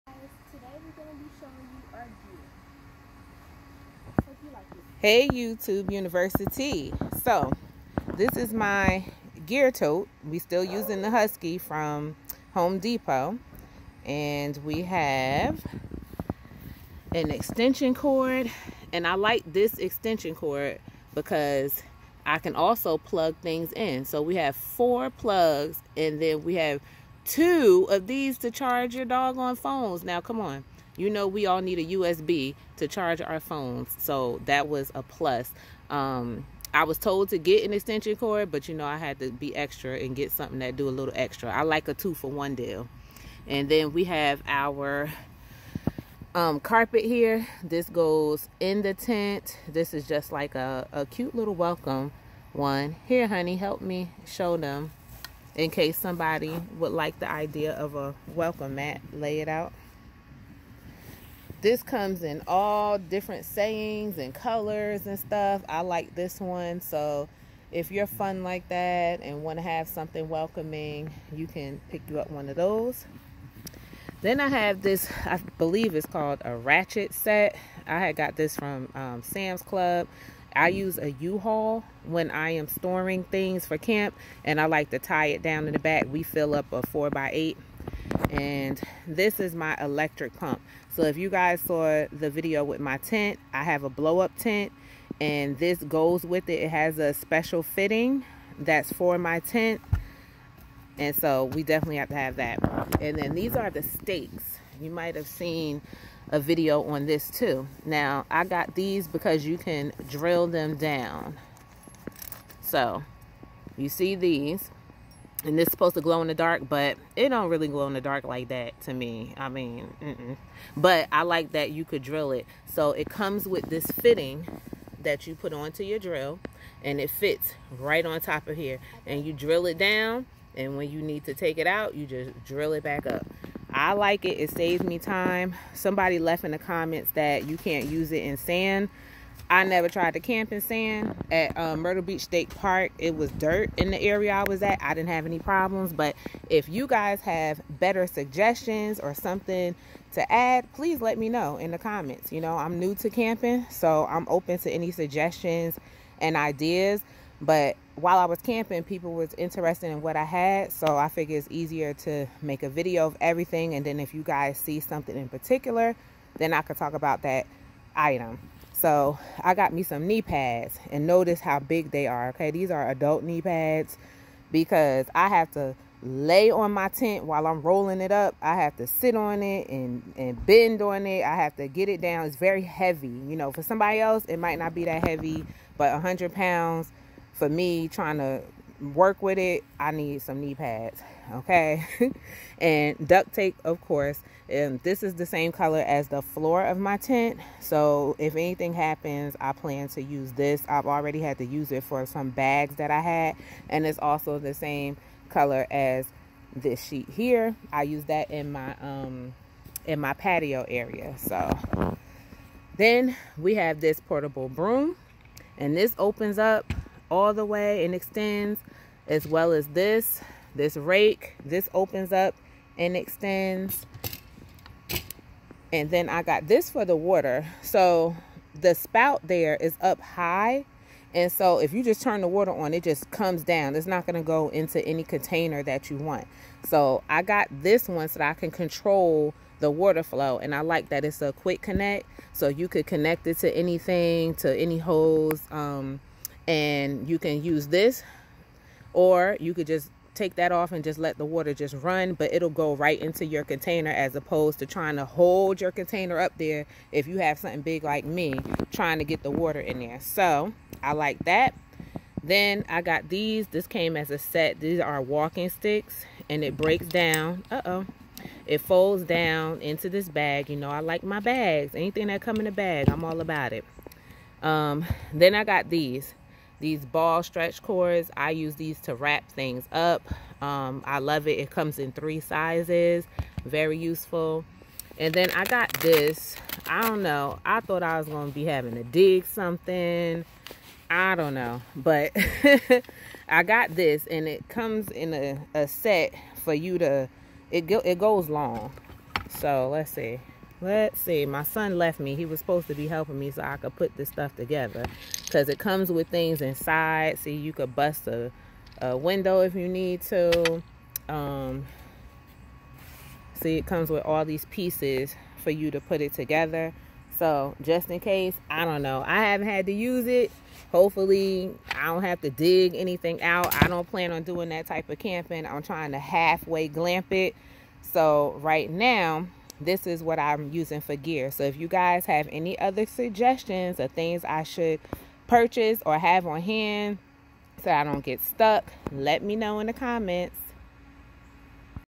Newbie Camping Gear | What's In My Gear Tote
Hey YouTube University🤩. Thank you for dropping by where I consult with you, the YouTube Alumni on my purchasing decisions😁

This video is an overview of the items I have in my gear tote. It's a work in progress and as I camp more, I'm sure I'll find more things to add. If you have something cool in your gear box, please let us know below 🥰

Here's what I have👇🏽

23 in. 50 Gal. Black Rolling Toolbox with Keyed Lock and Portable Hand Tool Tray

Masterplug Power At Work Four Power Outlets, Open Cord Reel with Winding Handle and Two USB Charging Ports, Overload Circuit Breaker and Power Switch, 50 Feet 14AWG, High Visibility Cord, Green
**They have 50ft, 75ft, and 80ft options with the USB. I have the 75ft in the video**

Znzd Camping Rules Vintage Decorative Doormat,Front Door Outdoor Entrance Farmhouse Campsite Porch RV Home Decor,Campers Camping Lovers Gifts,30 x 17 Inches Doormats
**This is one I have, but it's temporary out of stock. Here is another cute one from the same brand. They have a bunch of nice options.**👇🏽
Nogrit Rustic Camping Rules Door Mat Doormat 17x30 Inch Camping Door Mat for Home RV Cabin Outside Inside Front Door Entrance,Camping Gifts for Campers Camping Lovers Adventurers

CAT Ratchet Tie Down and Cam Buckle Set (4 pc.)

Outback Element® - 15 Tent Stakes Heavy Duty with Glow in The Dark Heads + Bonus Canvas Carry Pouch and Hex Drill Bit

Adventure Seeka 15 Pack - Super Heavy Duty Threaded Tent Stakes - Screw Style Glow-in-The-Dark Ground Anchor Pegs - Hex Head Driver and Convenient Storage Bag Included

Camco Broom and Dustpan for RVs, Adjustable from 24 to 52 Inches (43623-A)

Rake for Leaves, Garden Leaf Rake for Lawns Heavy Duty, 64 inch Adjustable Folding Metal Rake with Expandable Head from 8-23 Inch, 15 Tines, Shrub Rake for Gardening Yard Flower beds, Camping Rake

Sahara Sailor Unbreakable Tactical Shovel-180 Degree Folding Shovel-Camping Shovels - 23 in 1 Survival Gear and Equipment Multifunctional Camping Gear Survival Tools for Caping Hiking

HORUSDY 45 Pack Ball Bungee Cords, Includes 6”, 9”, 11” Heavy Duty Gazebo Tarp Tie Down Cord.

Wofeili Garden Retractable Hose Reel, Portable Simple Home Hose Reel 45+5 FT with 7 Patterns Spray Nozzle, Wall Mounted and Any Length Lock for Garden Watering, Car Washing, Pet (Red Rose)